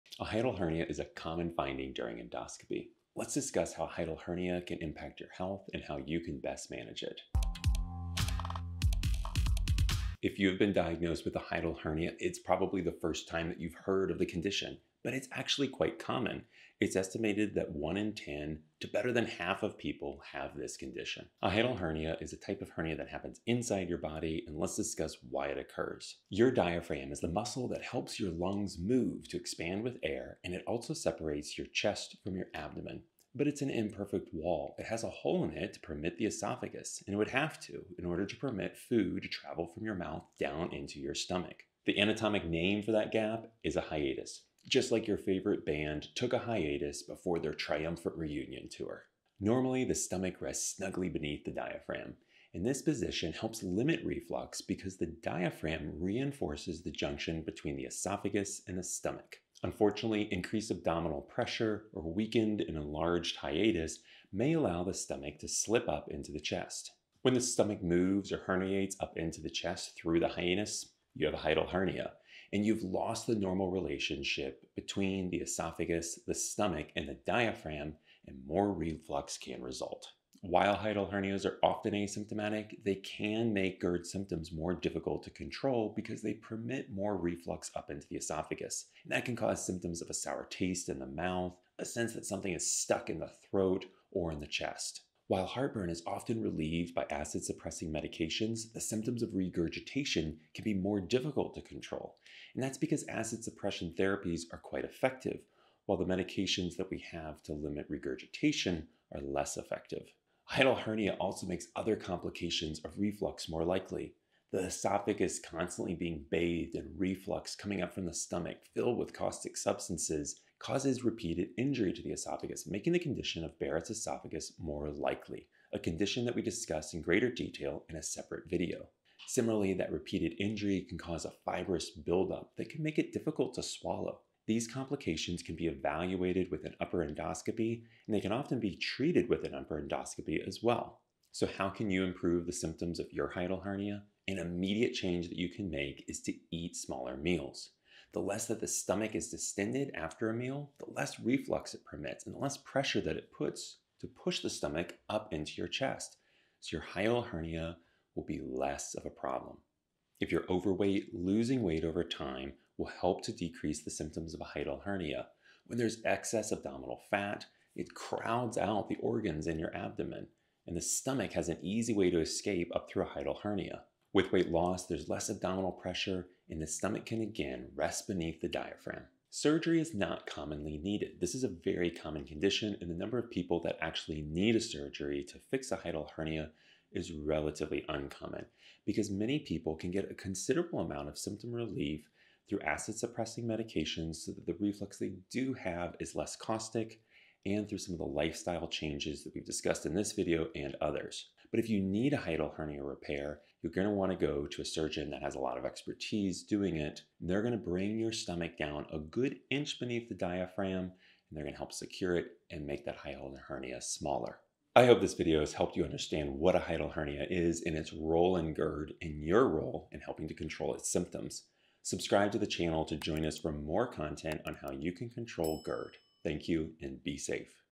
Hiatal Hernia- Symptoms, Complications and Treatment
Were you recently diagnosed with a Hiatal Hernia?  What are its symptoms, complications, and how should it be treated? Is a Hiatal Hernia making your GERD symptoms worse?

A Hiatal Hernia is a very common condition and in this video I cover many different topics surrounding it. Why does a Hiatal Hernia occur? What is the difference between the normal chest anatomy and the chest anatomy with a Hiatal Hernia? Why does a Hiatal Hernia make GERD symptoms worse? Why is regurgitation worse with a Hiatal Hernia? What are the complications of a Hiatal Hernia? How can you treat/improve the symptoms of a Hiatal Hernia? And do you need surgery for a Hiatal Hernia?

Watch this video to find out the answers to so many Hiatal Hernia questions!